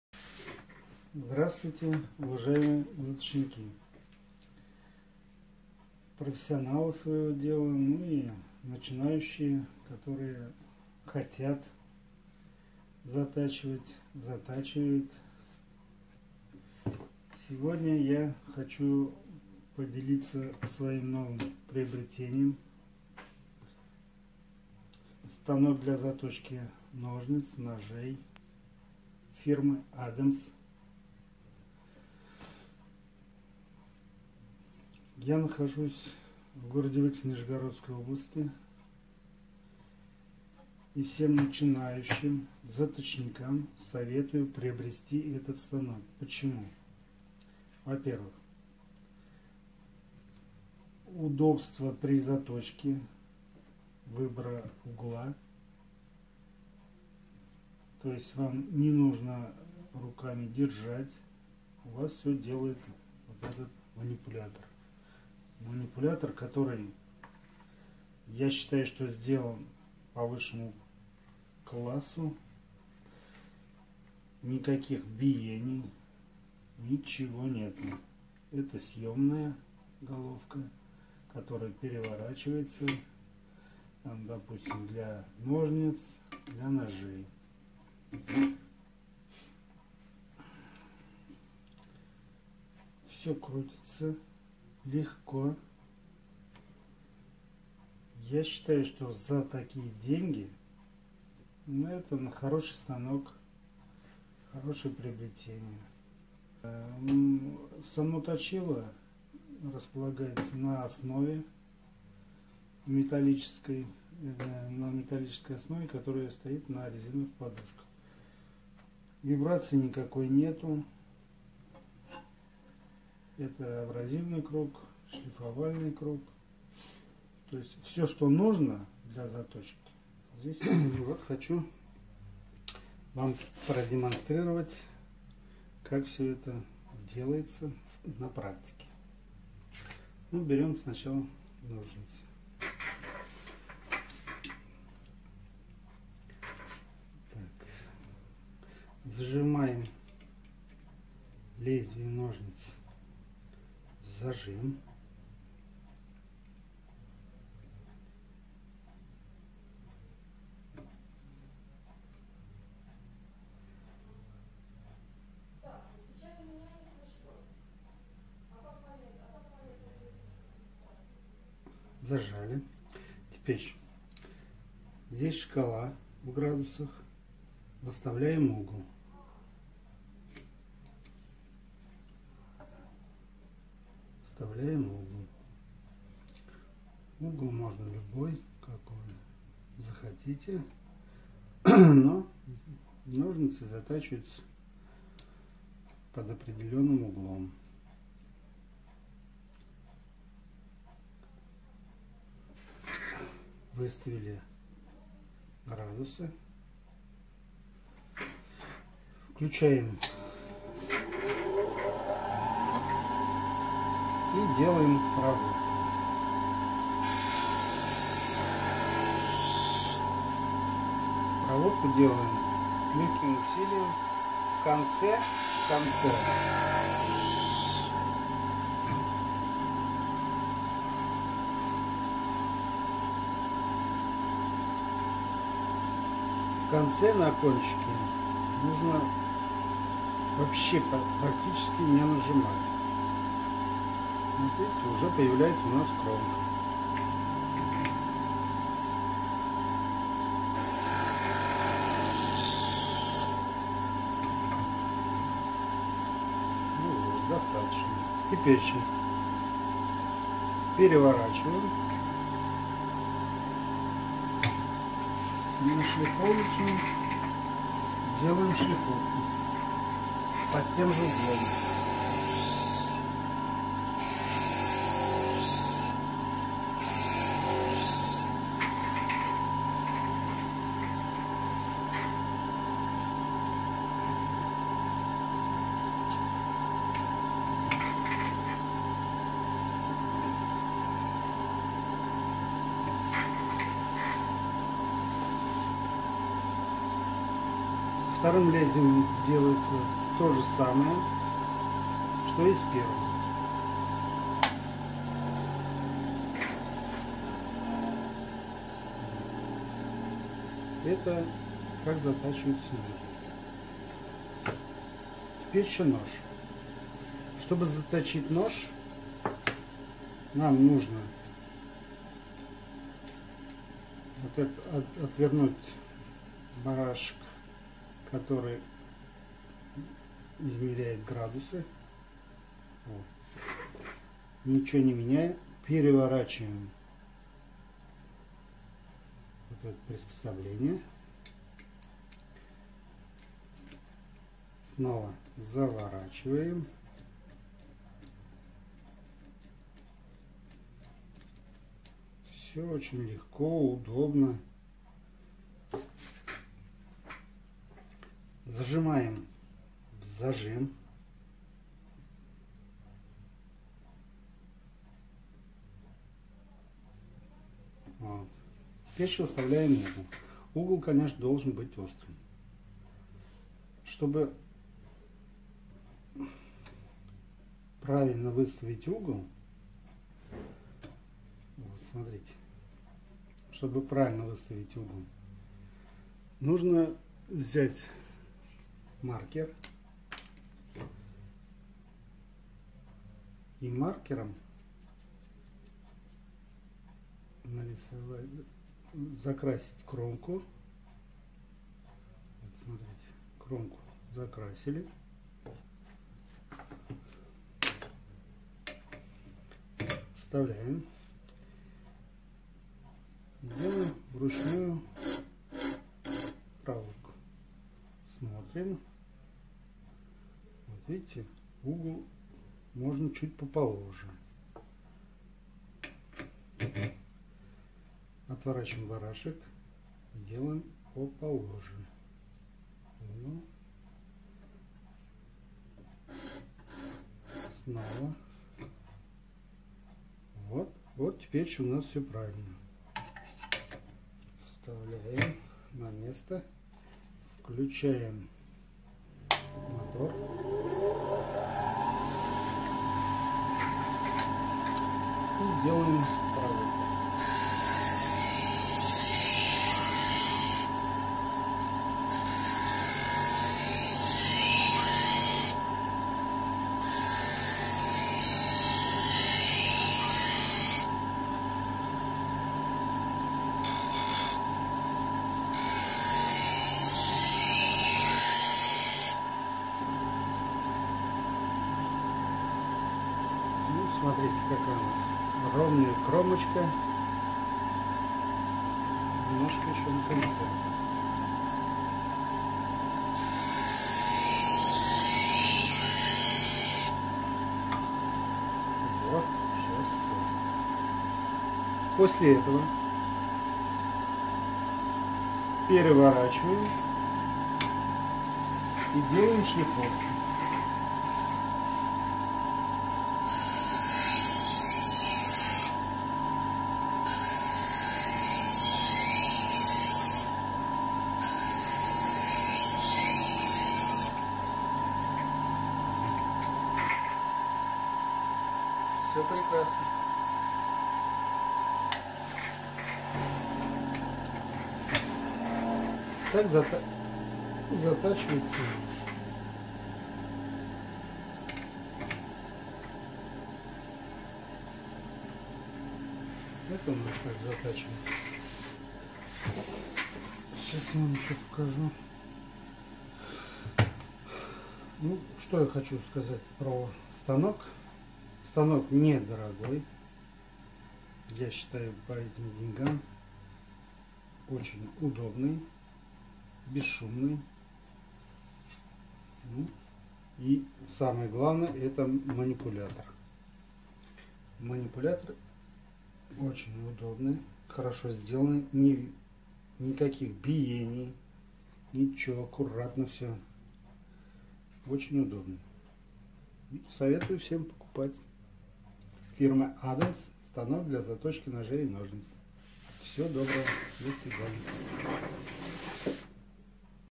Отзыв о заточке ножей и ножниц на станке ADEMS PRO
Сергей Сергеевич из города Выкса делится своими впечатлениями о заточном станке ADEMS PRO. В этом видео он довольно подробно рассказывает о станке, а также показывает заточку ножниц и ножа с помощью ADEMS PRO.

Узнай больше на adems.ru